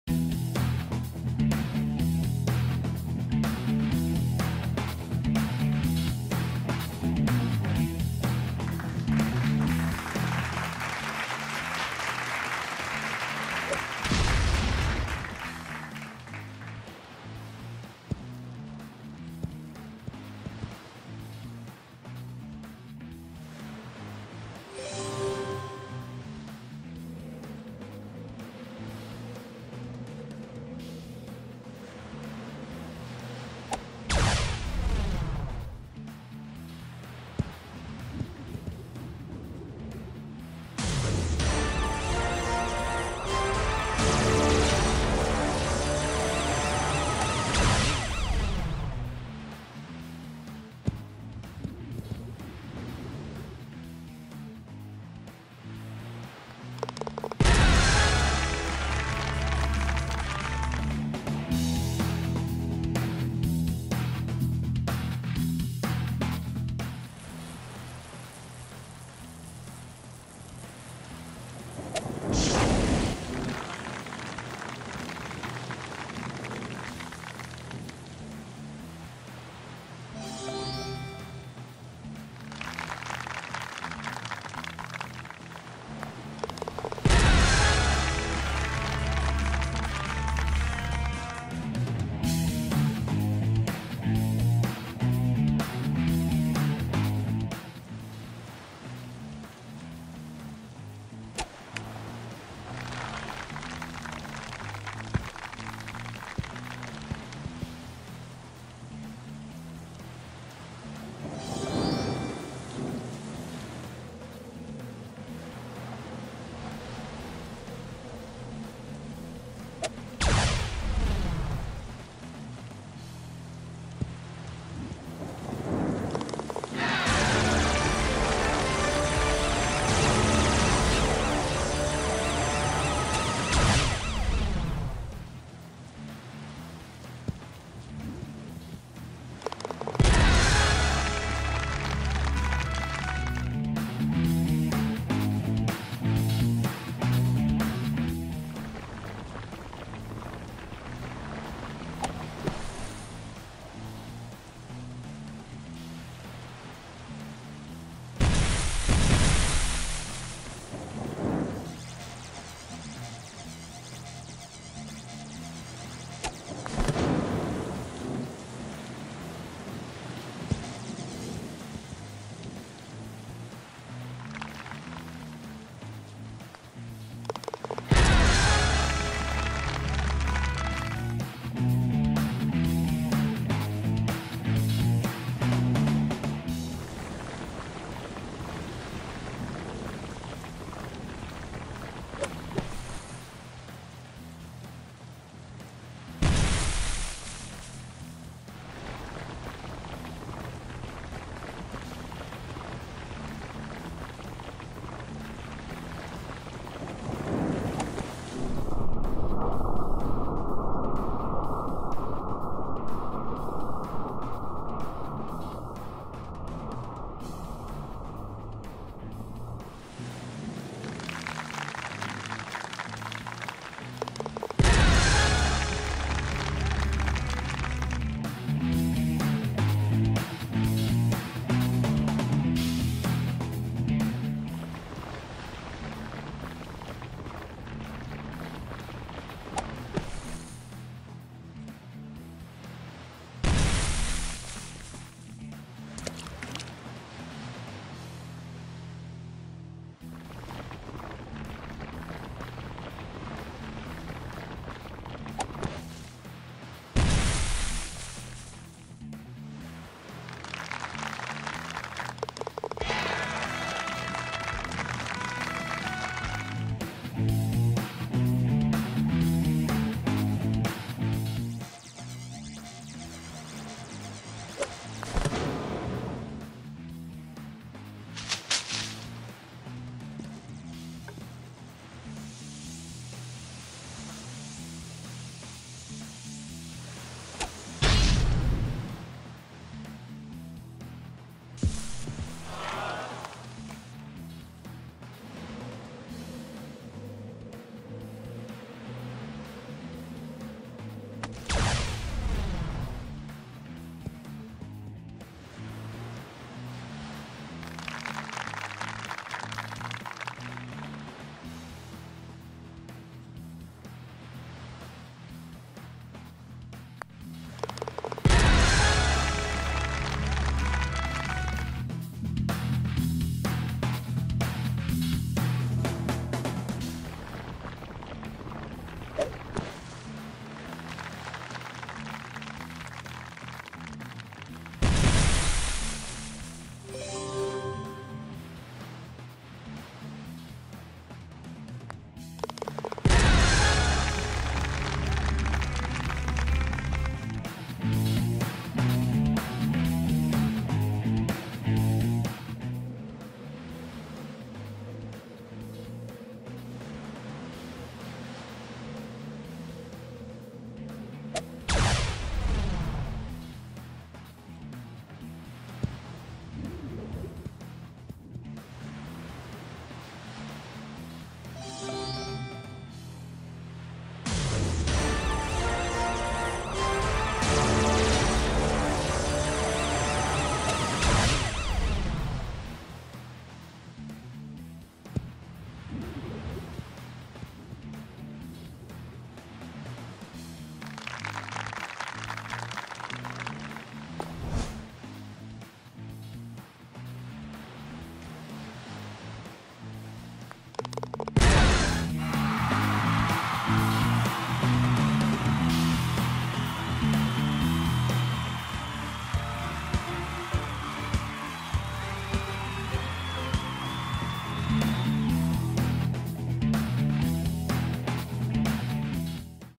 Silver Expert Friend Match (Front 9) (-10) vs WORZEL GUMMIDGE 3-07-2024
Played plenty today, best Output on Great Shot Points ,but the worst score.  Gotta take a break lol 😂